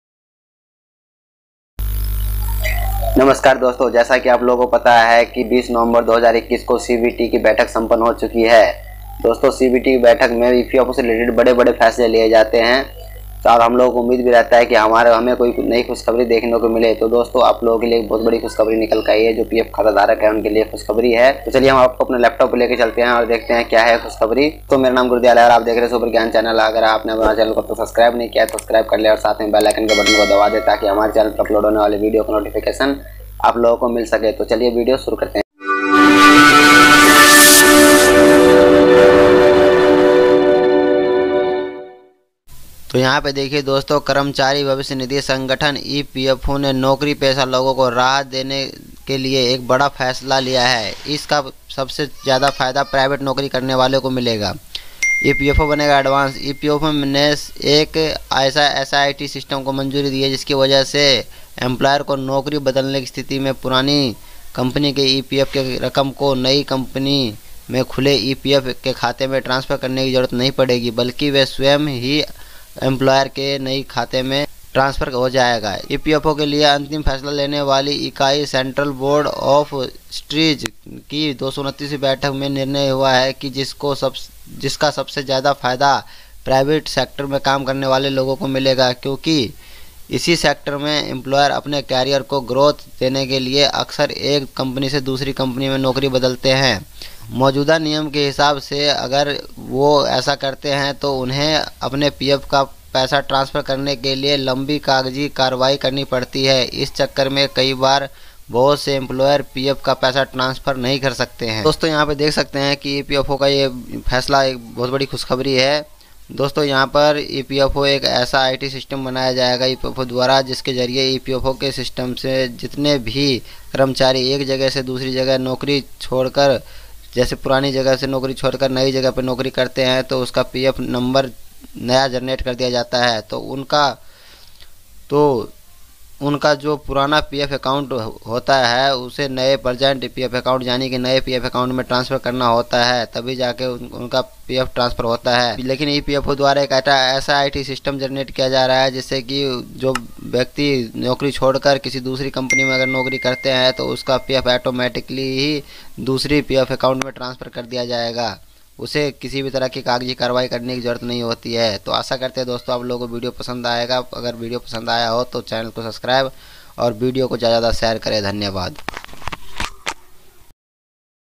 PF transfer big update 2021/ab PF ka Paisa transfer karne ki jarurat nahin/sabhi kam hoga aasan
About

namaskar doston Aaj is video ke madhyam se main aap logon Ko. EPFO balance transfer ke bare mein new update kya hai hai uske bare mein bataya hai. video ko pura dekhen agar video pasand aaye To channel ko subscribe jarur Karen dhanyvad.

PF Transfer New Update 2021
PF Transfer New Update
Kya ab PF transfer nahi karna padega 2021